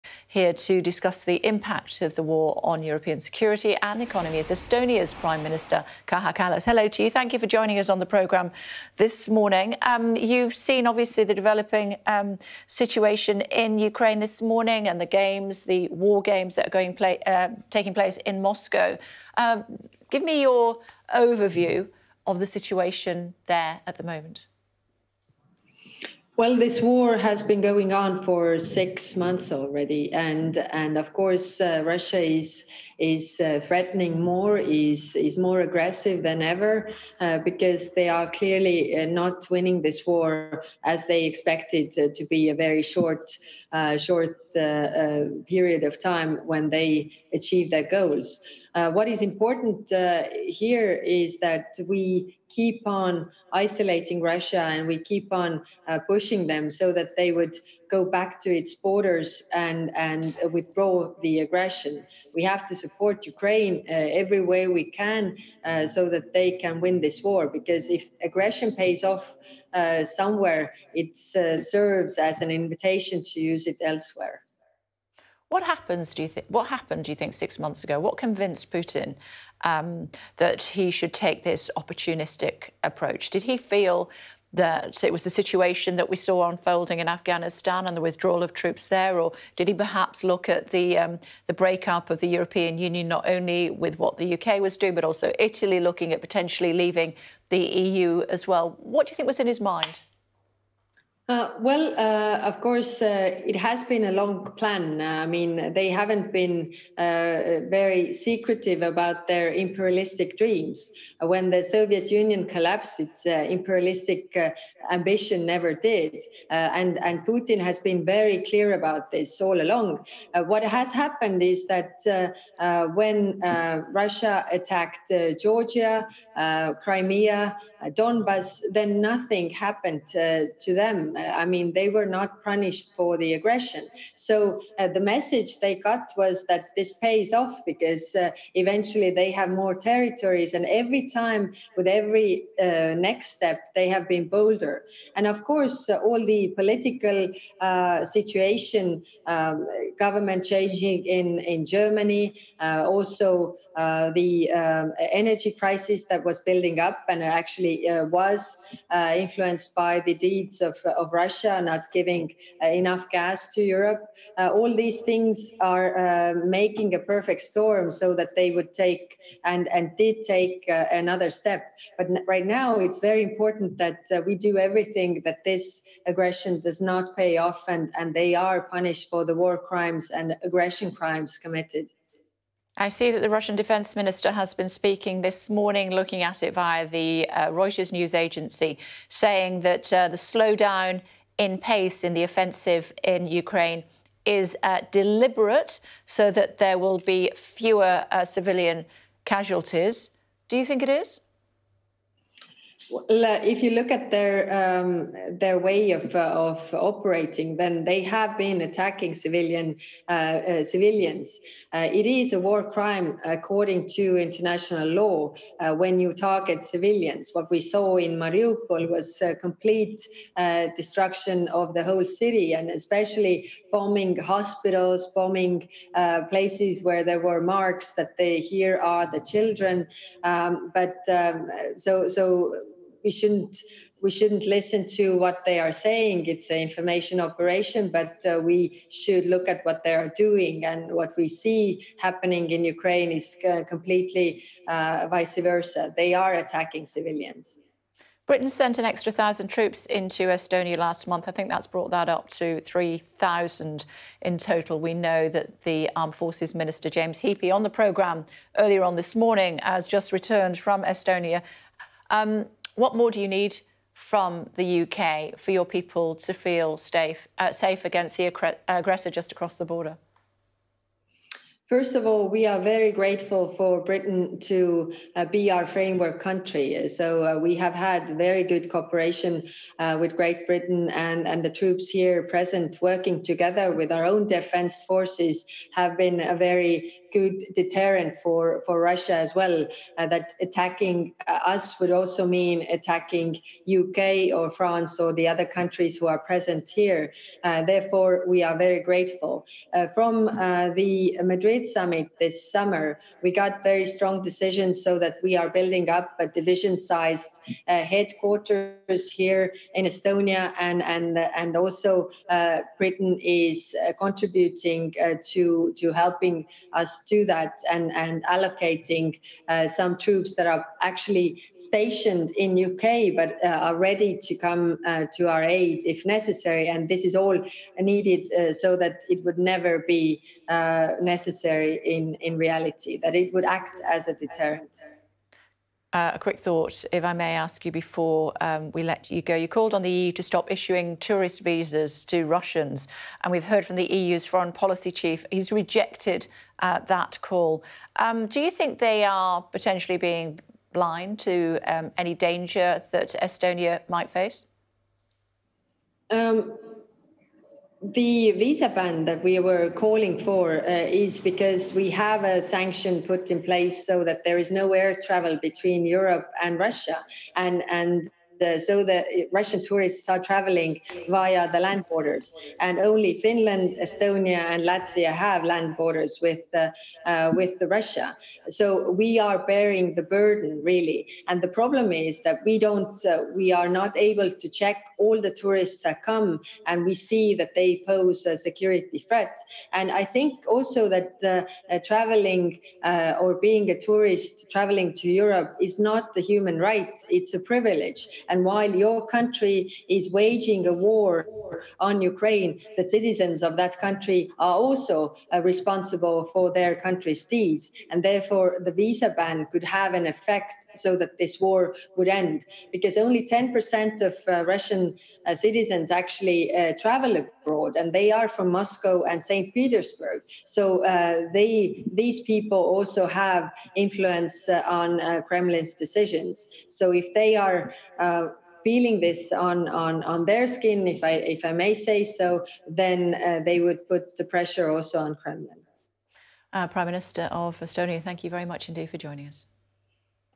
'Russian tourists entering Europe pose security threat' - Estonia's PM
Estonian Prime Minister Kaja Kallas has been speaking to Sky News after calls for an EU-wide visa ban for Russians were rejected, saying Estonia "is not able to check all tourists".

Russian travellers have been restricted to land travel due to air sanctions between Europe and Russia.

Only Finland, Estonia and Latvia have land borders with Russia.